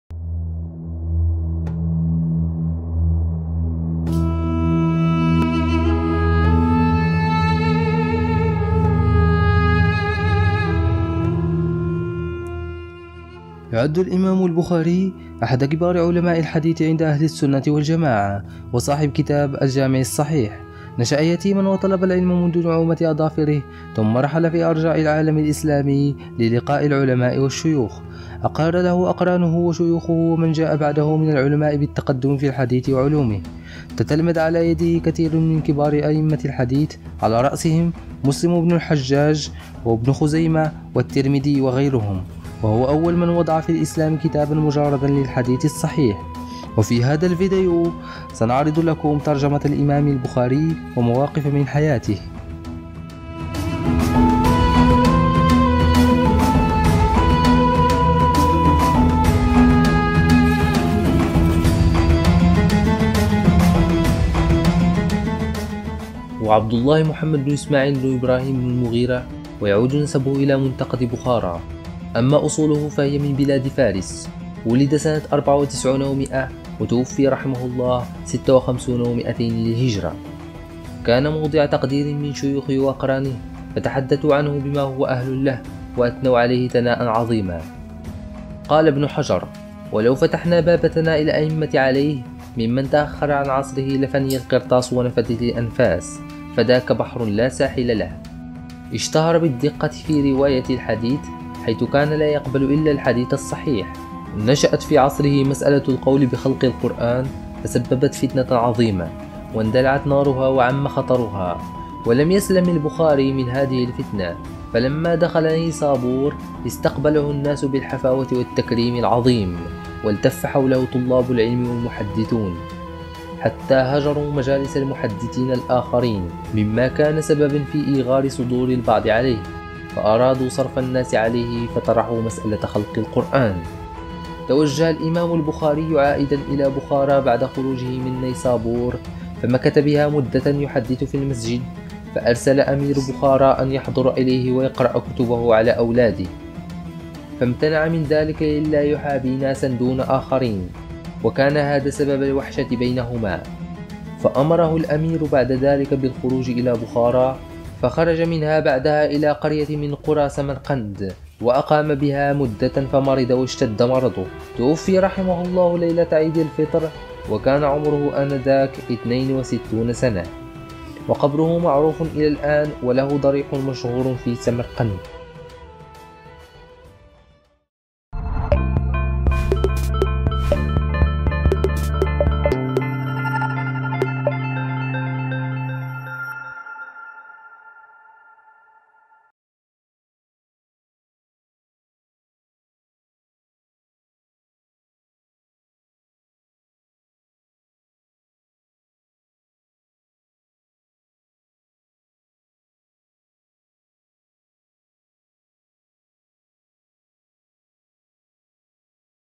الامام البخاري| أمير المؤمنين في الحديث و سيد الفقهاء | ترجمته و مواقف من حياته
يعد الامام البخاري أحد كبار علماء الحديث عند أهل السنة و الجماعة و صاحب كتاب الجامع الصحيح